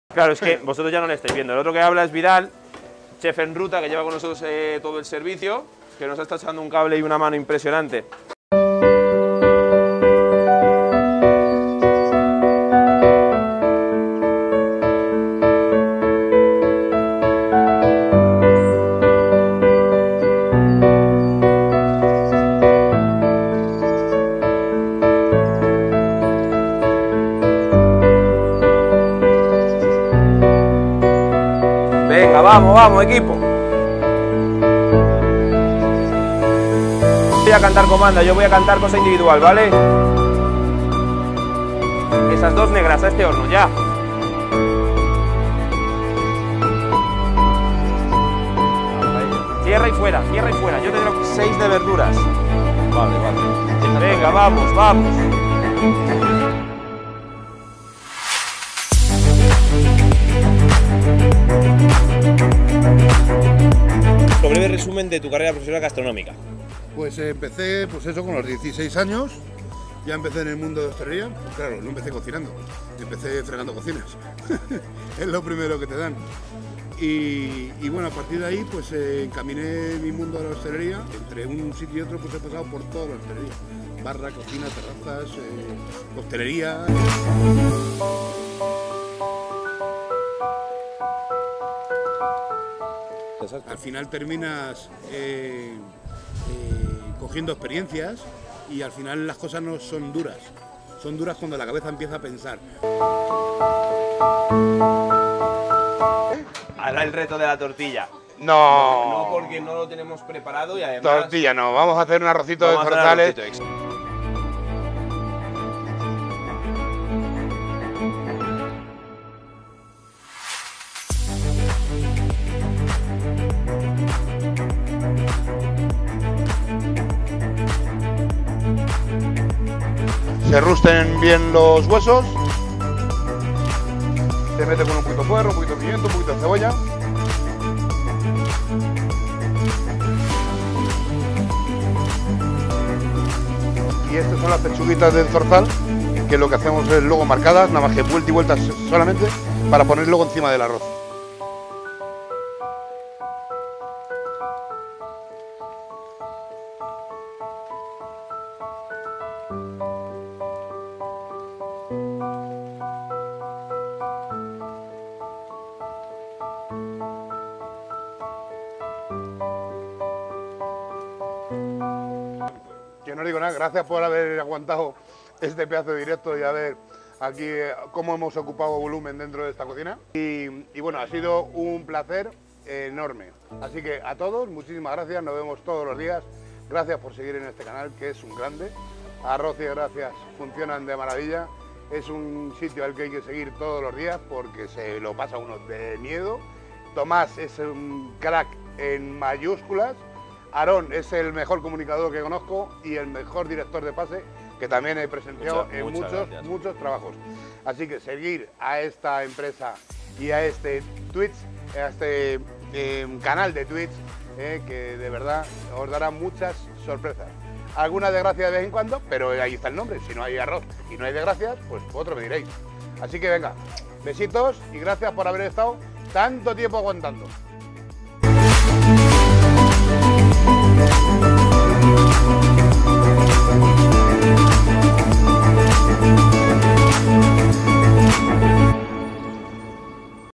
Chefenruta en la cocina de Arroz y Desgracias | Mejores Momentos
¿No viste el stream de Arroz y Desgracias en Twitch el día que vino a visitarnos y ayudarnos en el servicio el grandísimo @CHEFENRUTA ? ¡¡¡Noooo te preocupes!!! 🤘🤘

Aquí te dejamos un breve resumen con los mejores momentos del stream, esperamos que lo disfrutéis tanto como nosotros lo hicimos ese día. 🔥🔥🔥